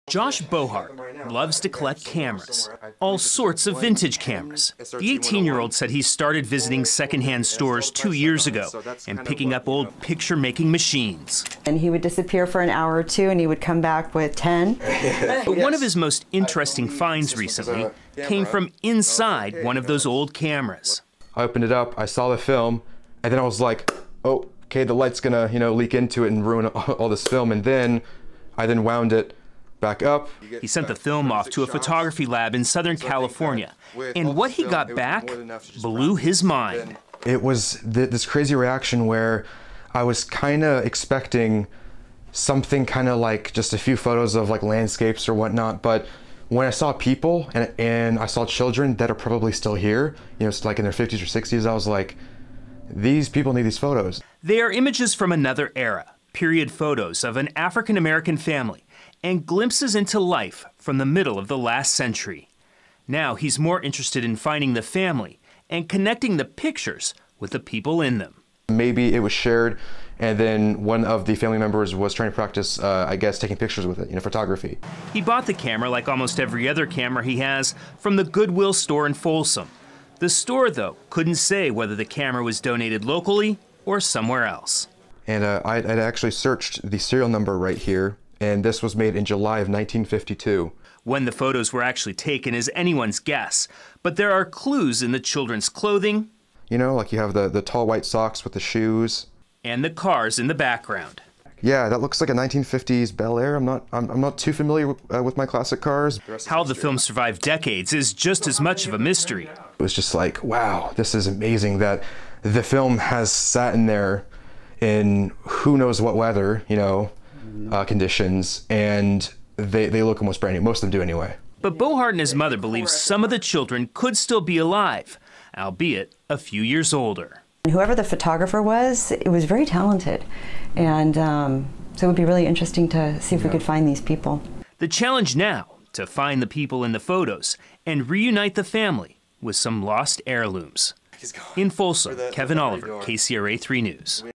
Folsom teen searches for family captured in vintage photos
A discovery at a thrift shop has led to a search for a family captured in a series of photos. A teen who found the vintage pictures is trying to return them to the family. The period photos show an African-American family and their life in the middle of the last century.